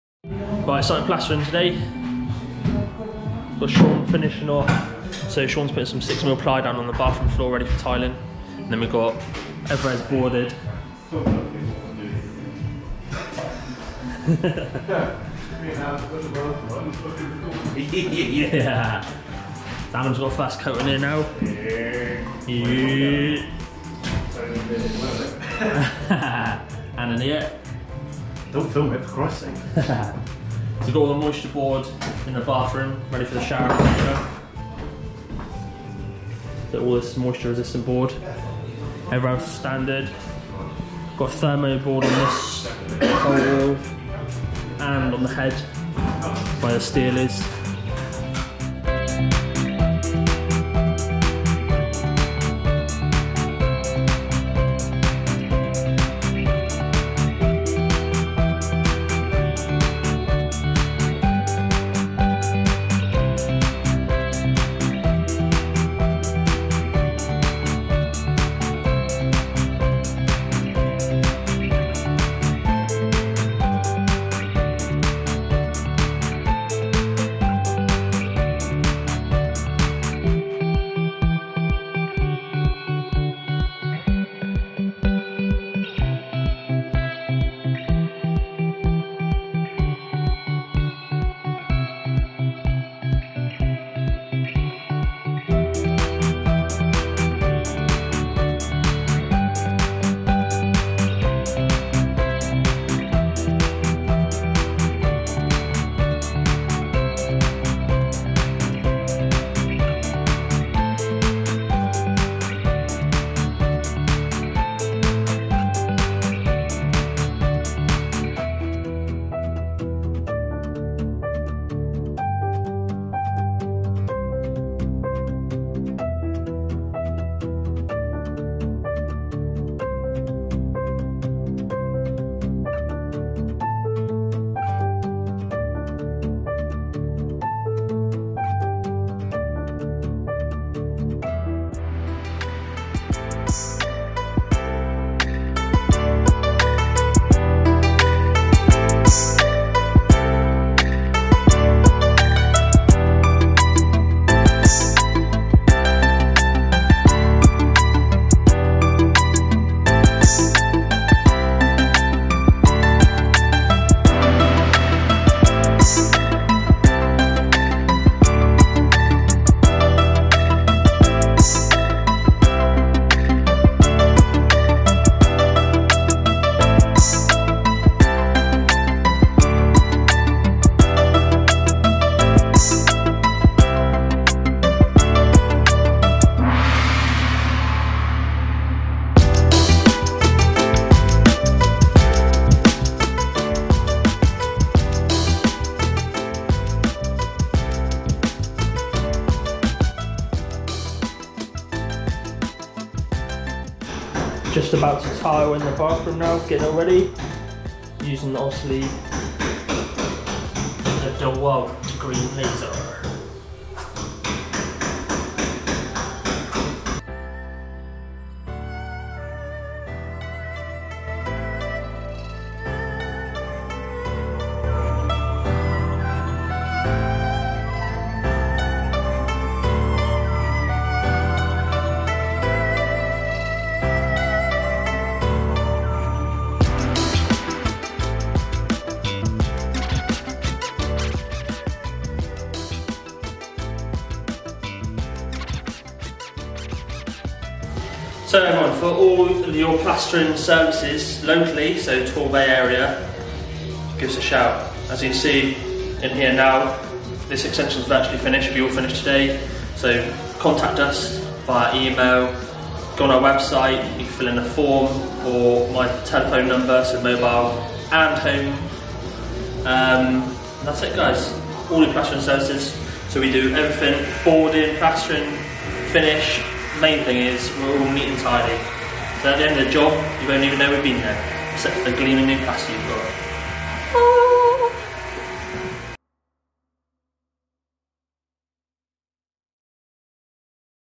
TDD Construction Project | Plastering an extension | Mixing Plaster | plastering walls and ceilings
In part 8 of this extension, in this TDD Construction project, you'll see where the TDD lads have plasterboards and plastered the inside of the extension, including moisture board in the on-suite bathroom.